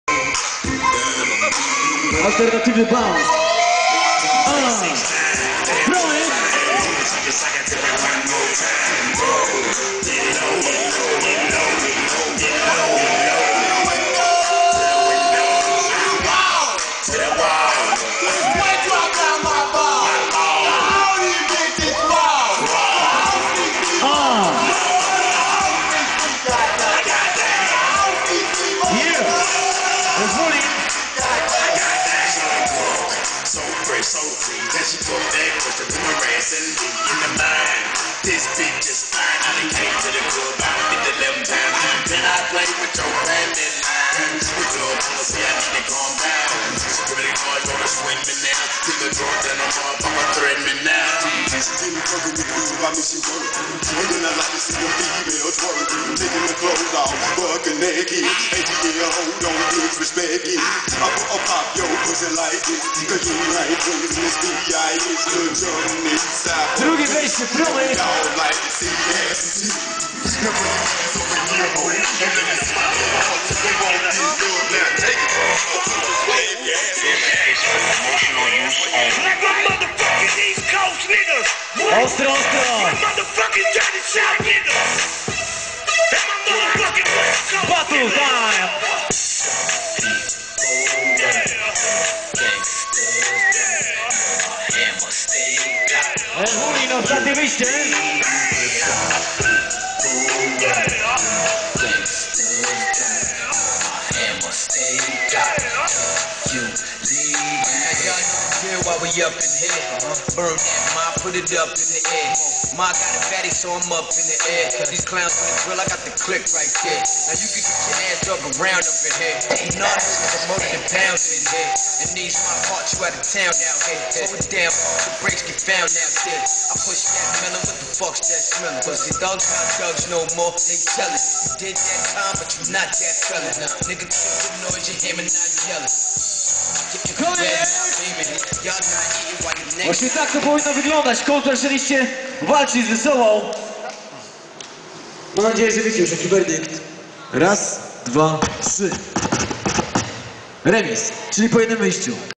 CHECK THE FLOW vol.3 Ewulin TST vs Płomyk
CTF vol.3 w Teresinie. Piękne dzięki i szacunek dla tancerzy, jurorów, prowadzącego i DJ-a. Peace!